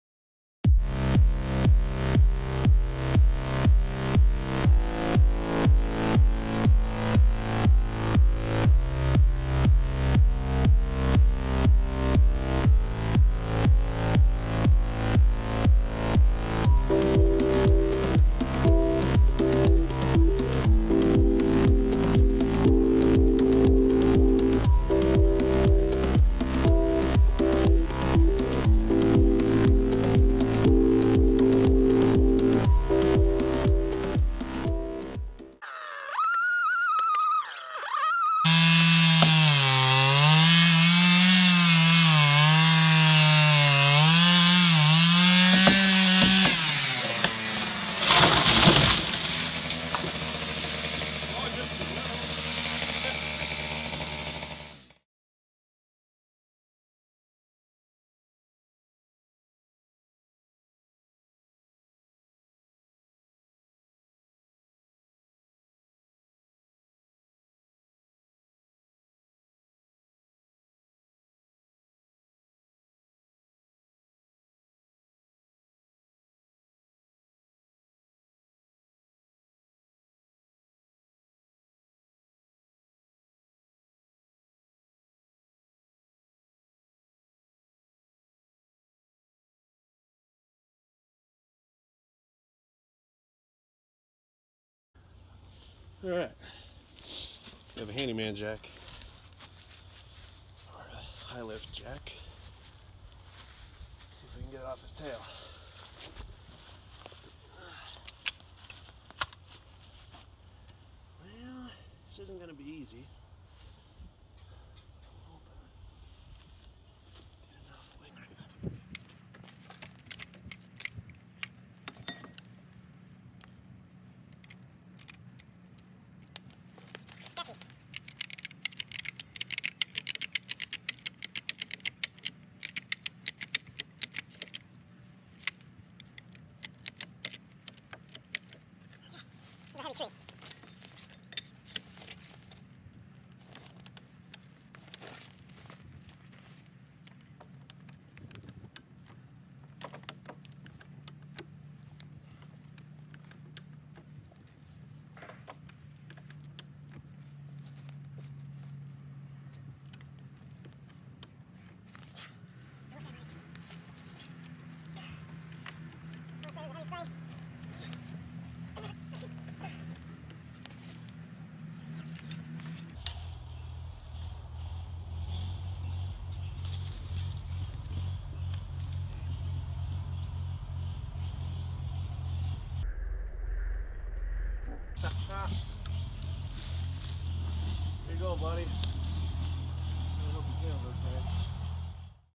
Squirrel extraction HD
A more detailed video showing how we got him out.